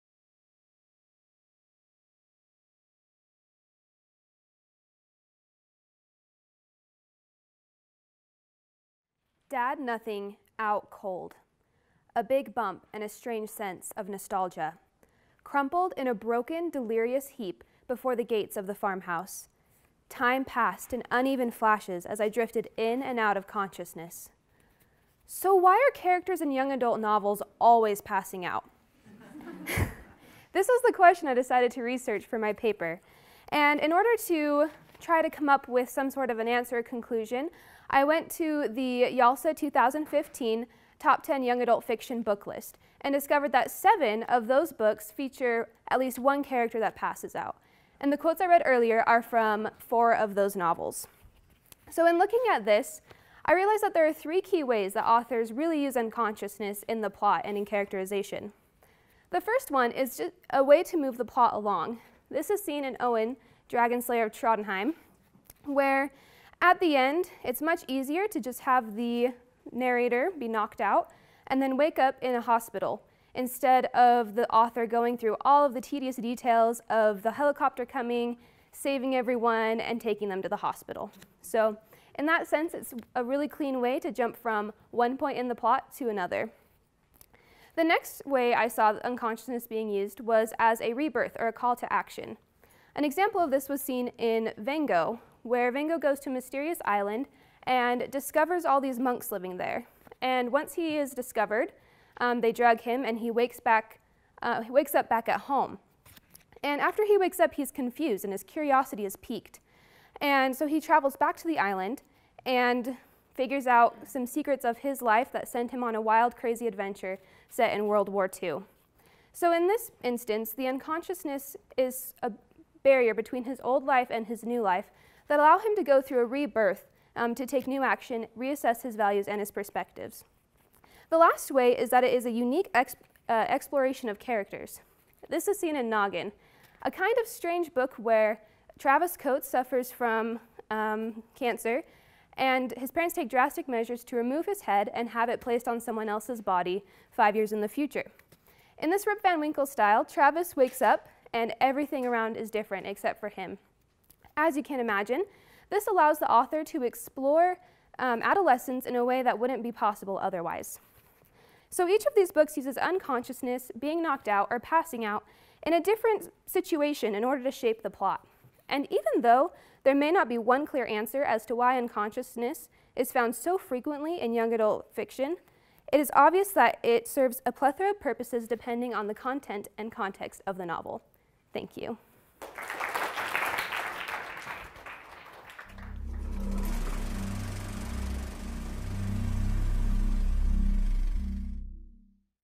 “Concussions as a Trope: Passing Out in Young Adult Literature” - Heidi Grether
A 3MP (three-minute paper) presentation given at the 2016 BYU English Symposium on Thursday, March 17, 2016 by Heidi Grether, based on her paper, “Concussions as a Trope: Passing Out in Young Adult Literature.”